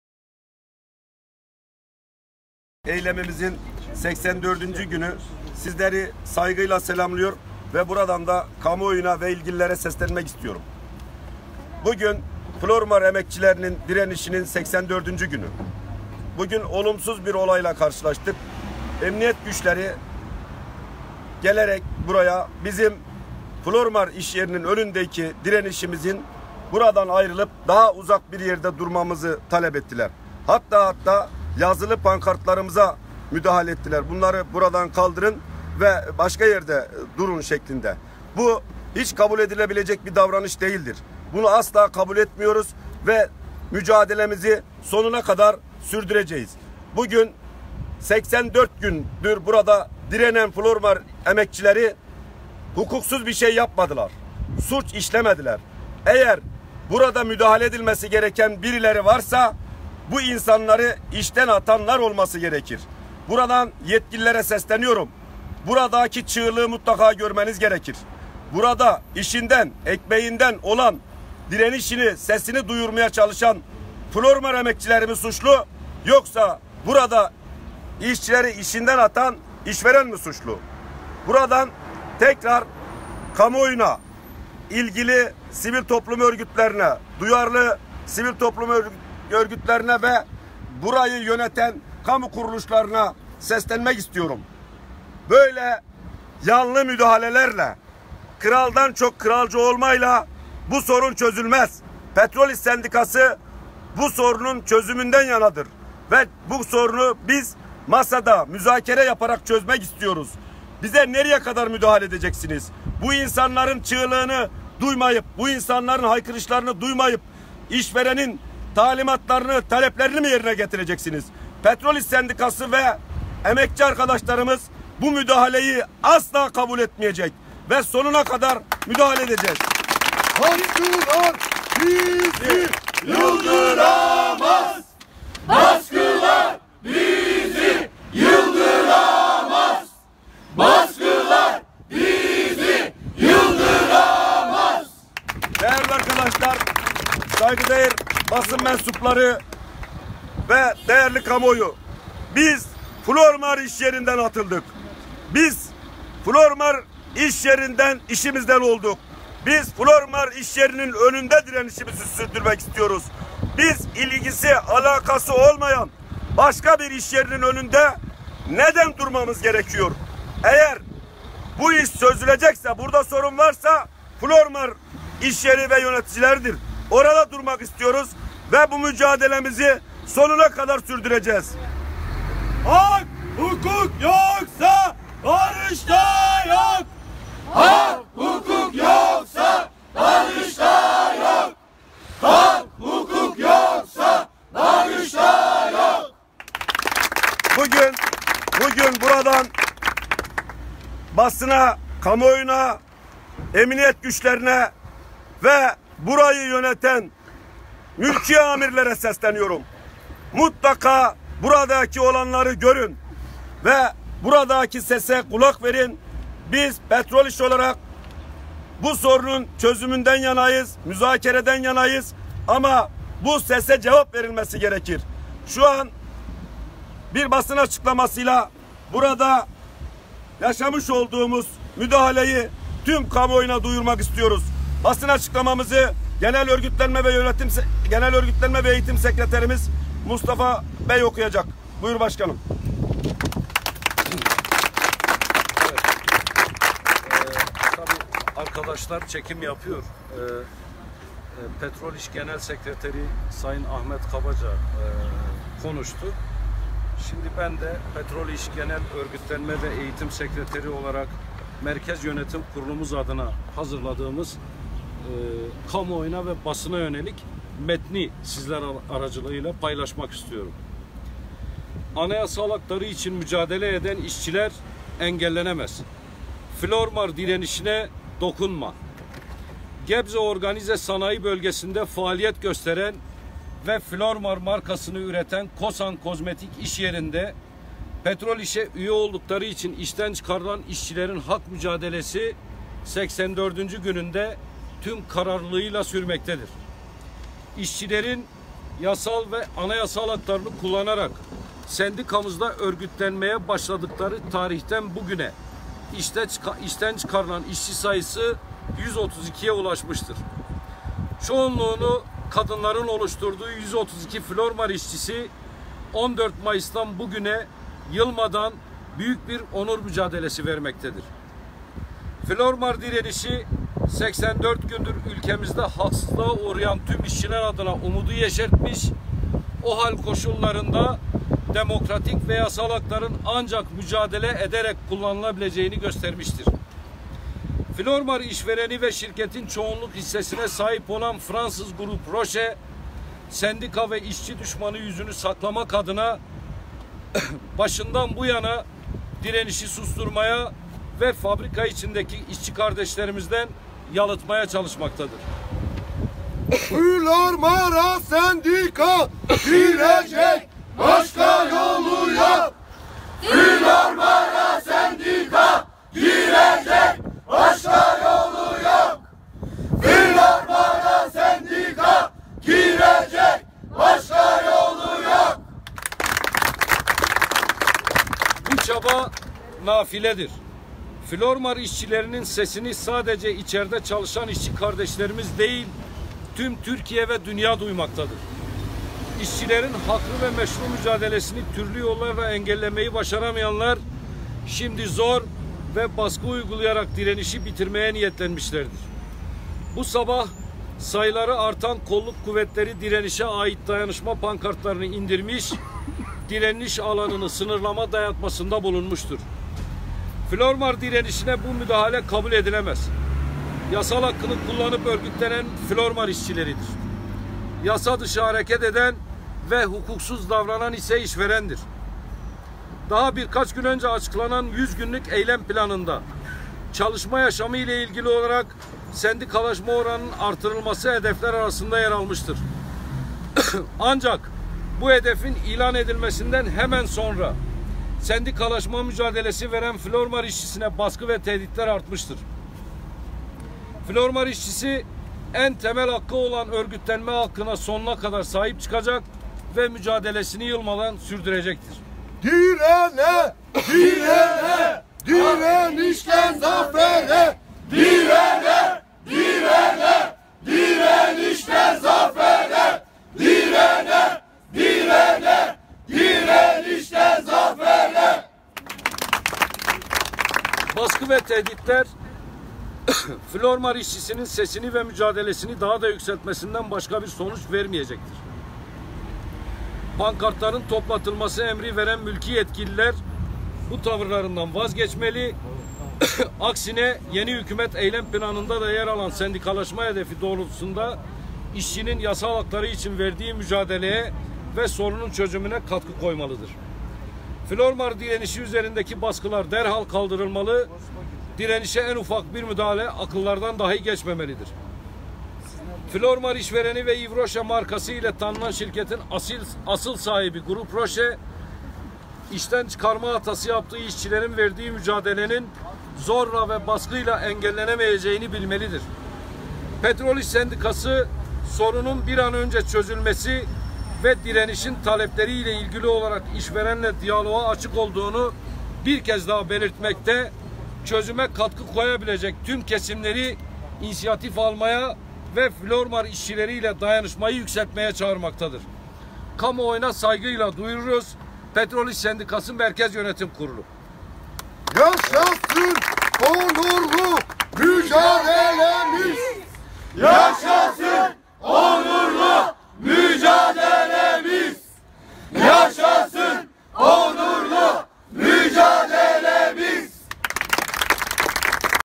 Artan Baskılara Karşı Flormar Önünde Basın Açıklaması Yapıldı
Genel Sekreterimiz Ahmet Kabaca ve Genel Örgütlenme ve Eğitim Sekreterimiz Mustafa Mesut Tekik, Flormar işçileri ile birlikte artan baskılara karşı bir basın açıklaması düzenlediler.

Genel Sekreterimiz Ahmet Kabaca ve Genel Örgütlenme ve Eğitim Sekreterimiz Mustafa Mesut Tekik, 6 Ağustos Salı günü Gebze Organize Sanayi bölgesinde faaliyet gösteren ve Flormar markasını üreten Kosan Kozmetik işyeri önünde basın açıklaması yaptılar. Direnişin 84. gününde, fabrika önündeki mücadelelerini sürdüren işçilere karşı, kolluk kuvvetleri tarafından pankartların indirilmesi ve direniş alanının sınırlama dayatmasına karşı Genel Sekreteimiz Ahmet Kabaca işçilere seslenerek, “Flormar işçilerinin suç işlemediğini, asıl suçu işverenin işlediğini” dile getirerek, “her zaman Flormar mücadelesinin yanlarında olacağını” belirtti.

Genel Sekreterimiz Ahmet Kabaca'nın ardından söz alan Genel Örgütlenme ve Eğitim Sekreterimiz Mustafa Mesut Tekik basın açıklamasını okudu.